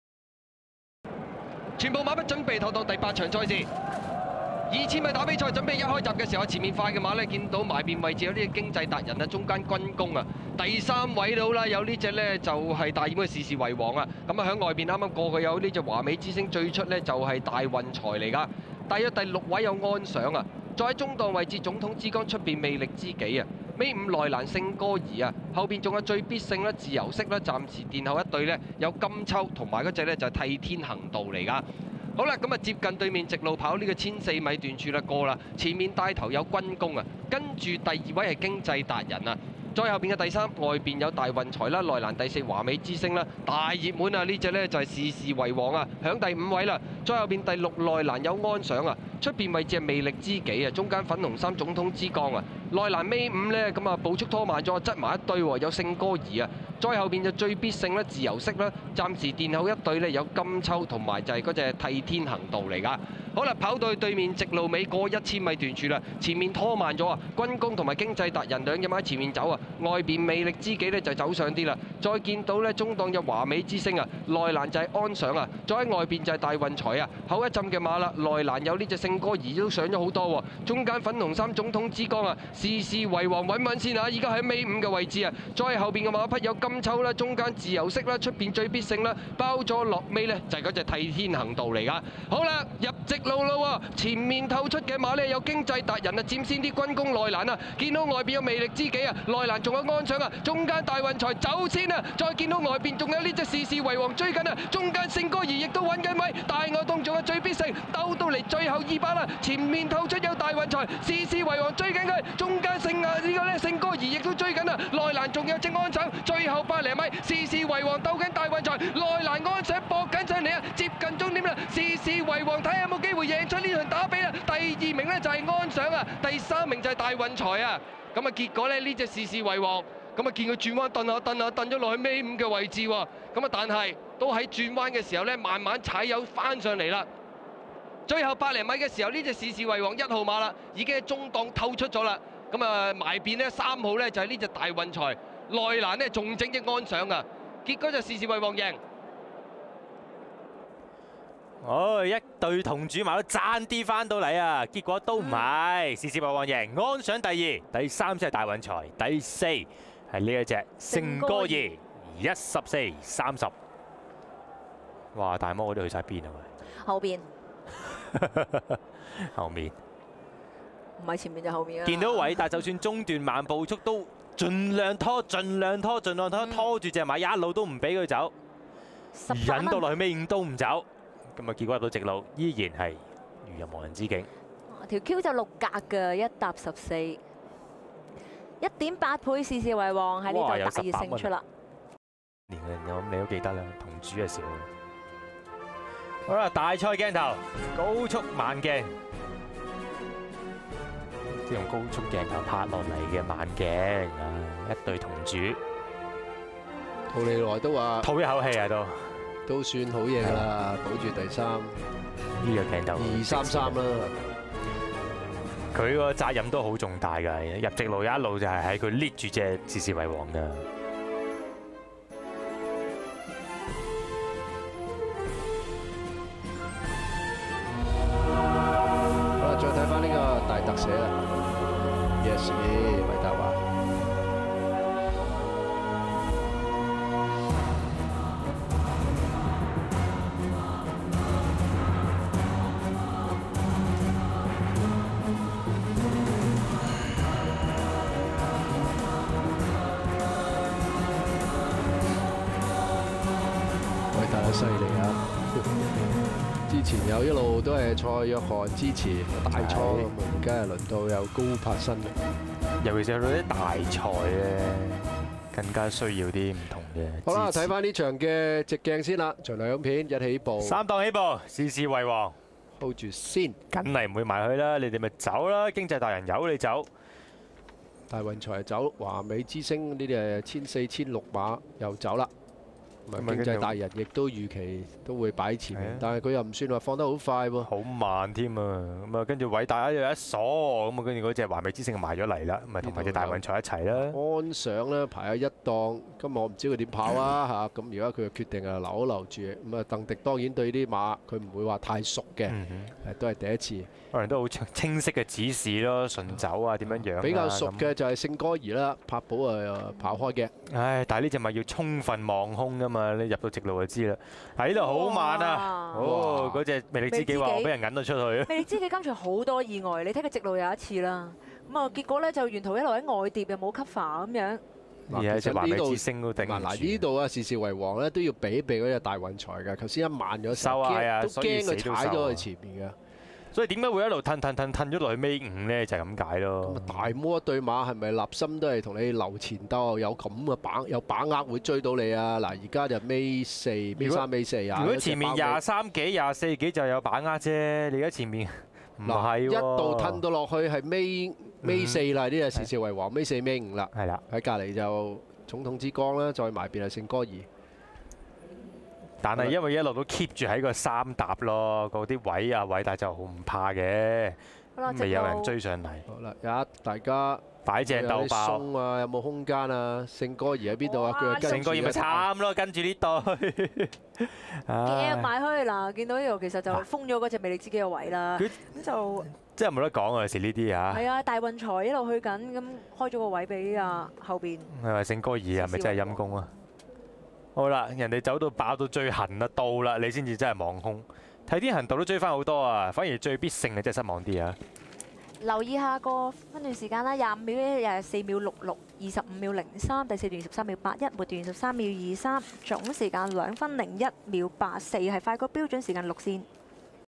17/03/2013 香港打吡 Hong Kong Derby 香港ダービー (468) 事事為王 (P125) 韋達
17/03/2013 沙田 (場次468)
場地狀況 :  好至快地
賽道 :  草地 - "A"
時間 :  (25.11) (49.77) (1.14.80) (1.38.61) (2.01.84)
分段時間 :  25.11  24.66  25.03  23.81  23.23